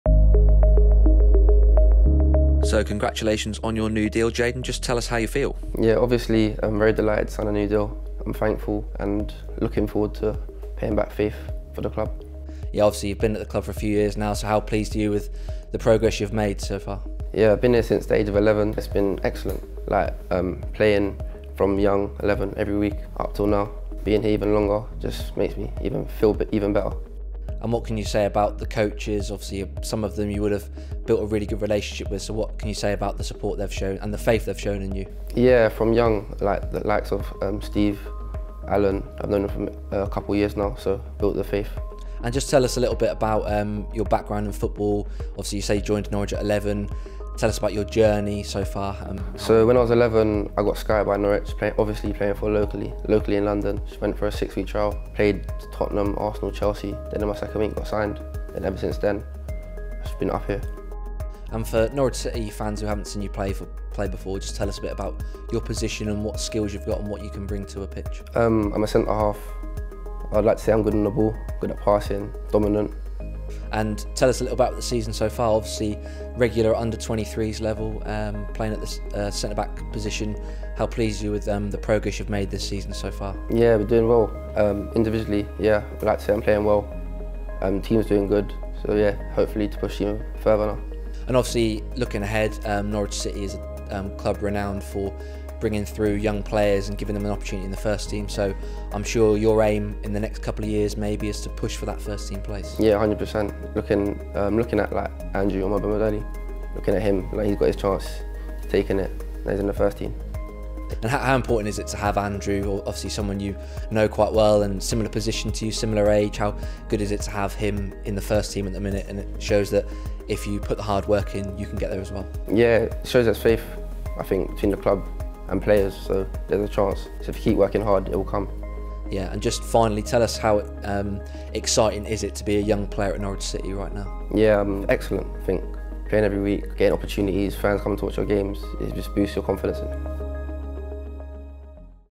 "IT'S EXCELLENT TO BE AT NORWICH!" | Under-23s defender Jaden Warner signs new deal
Hear from Under-23s defender Jaden Warner after he signed a new contract with Norwich City.